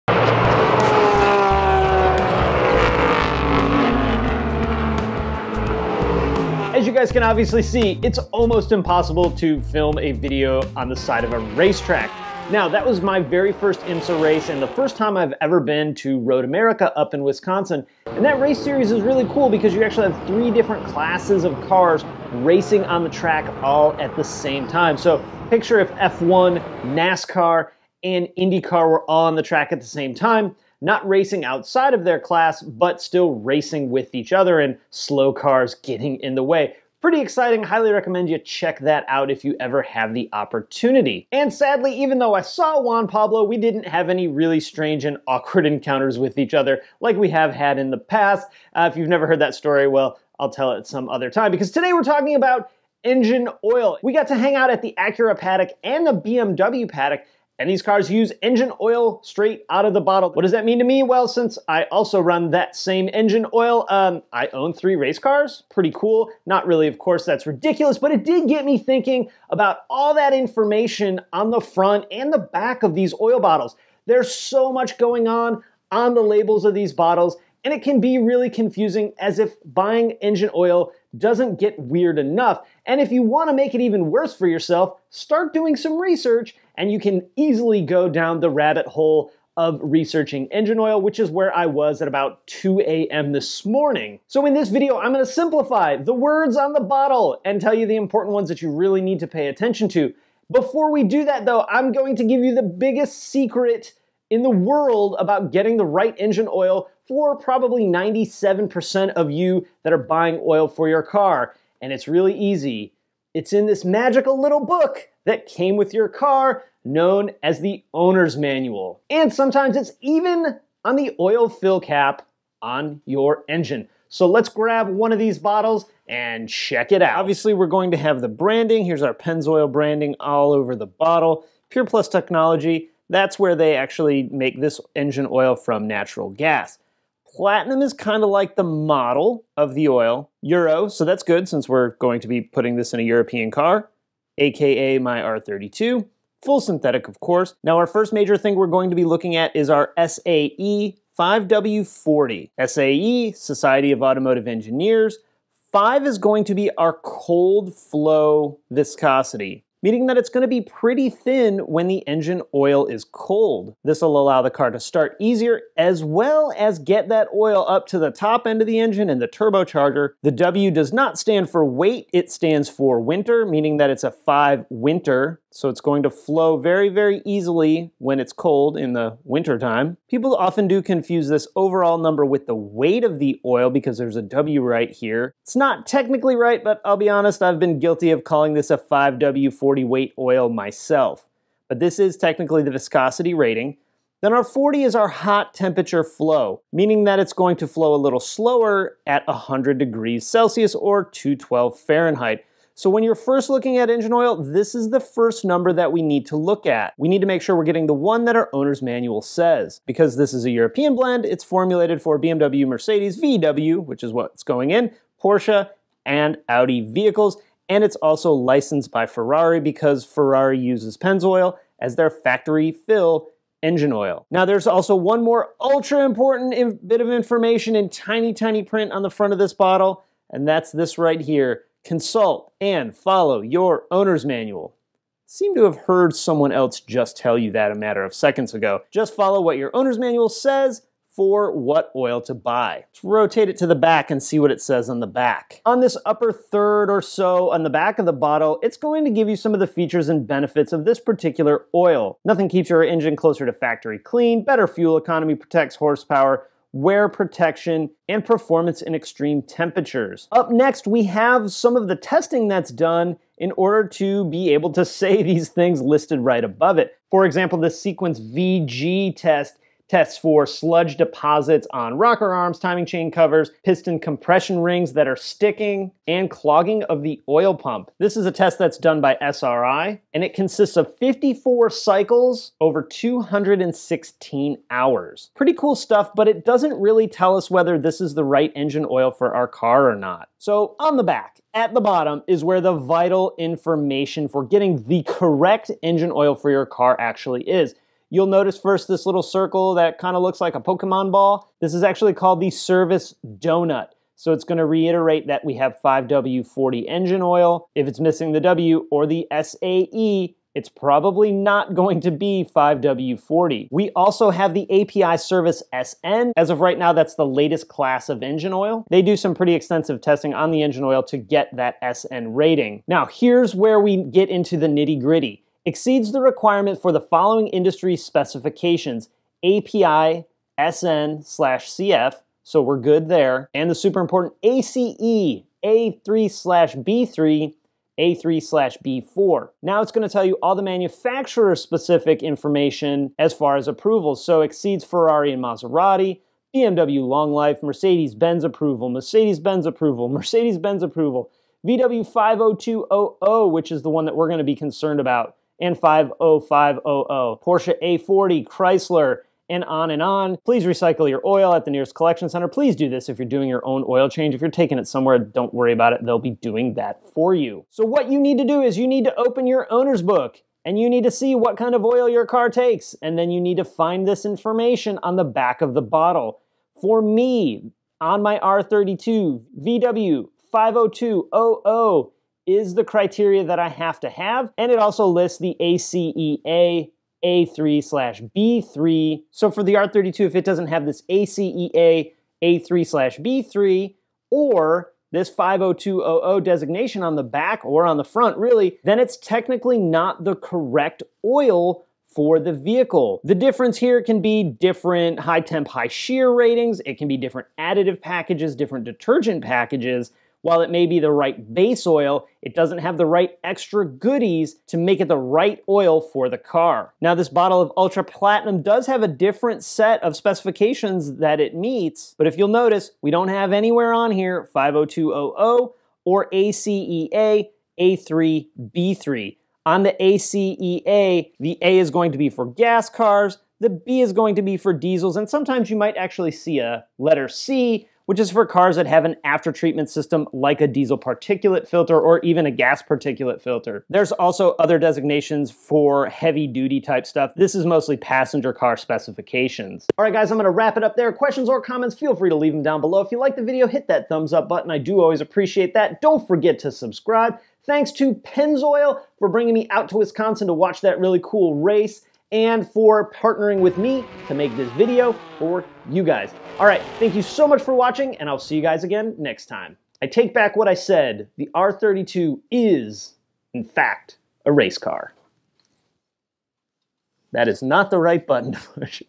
How to Read Engine Oil Bottles ~ Always Get The Right Oil
Buying the right engine oil can be confusing. But it's actually very simple. Using the information listed on the engine oil bottle, and your owners manual, you never have to worry about getting the wrong engine oil for your car or truck. Reading the engine oil bottle can be tricky. Here are the simple facts! Thanks to Pennzoil for partnering with me on this video.

Oil in my R32 ~